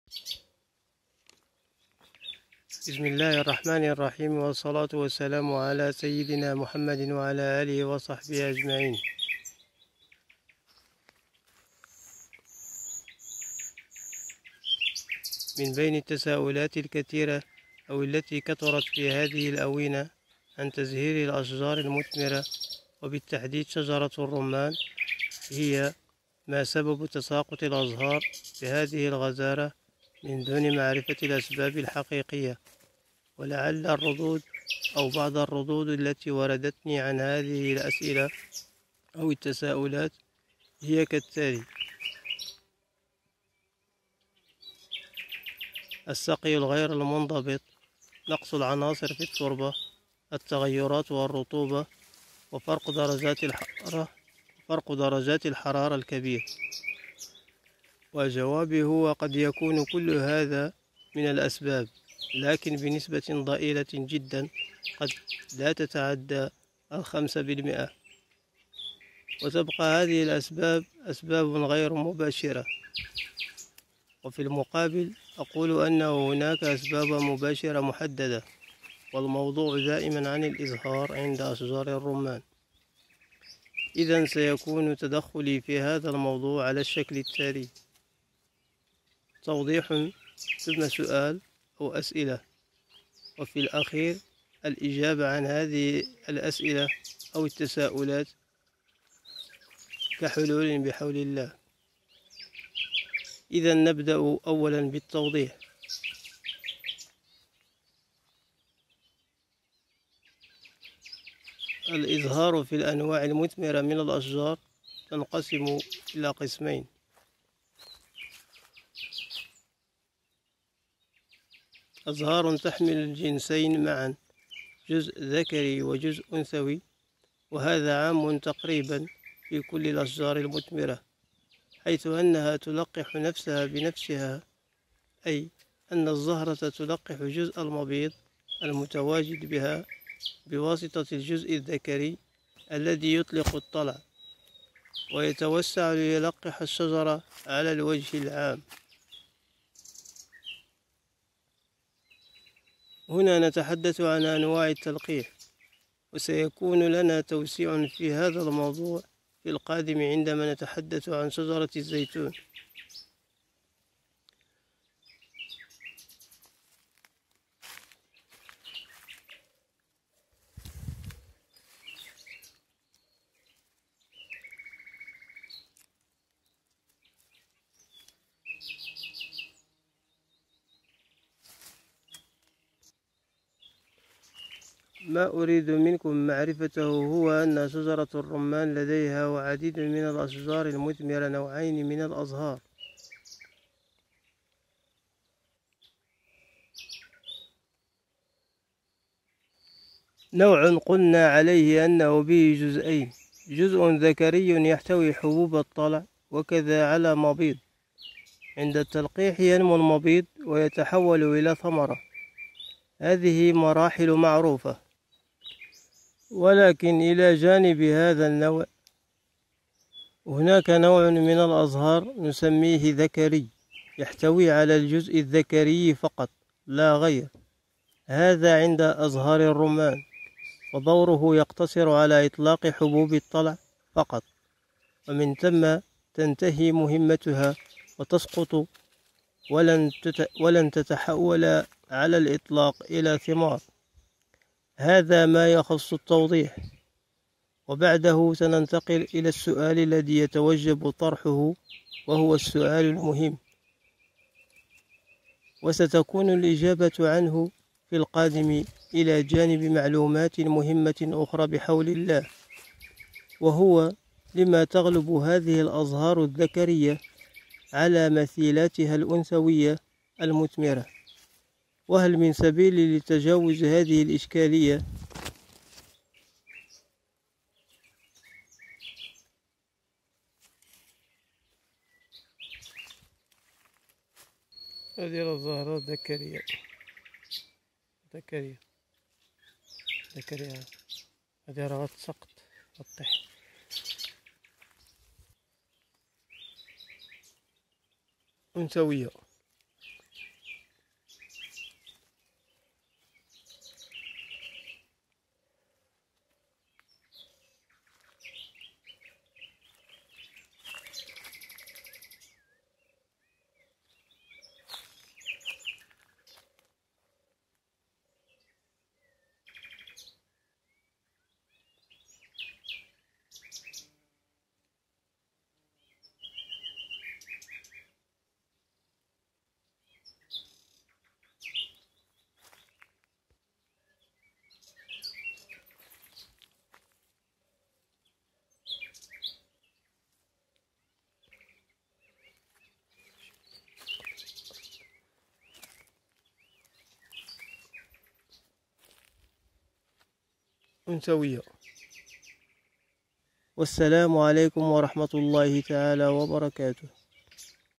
أسباب تساقط أزهار الرمان بكثرة
اسباب تساقط ازهار الرمان مع الحلول تساقط ثمار الاشجار المثمر كيفية المحافظه على الاثمار و سبب عدم تكون الازهار المثمرة السبب الحقيقي وراء ظهور الازهار الذكرية و الحلول لذالك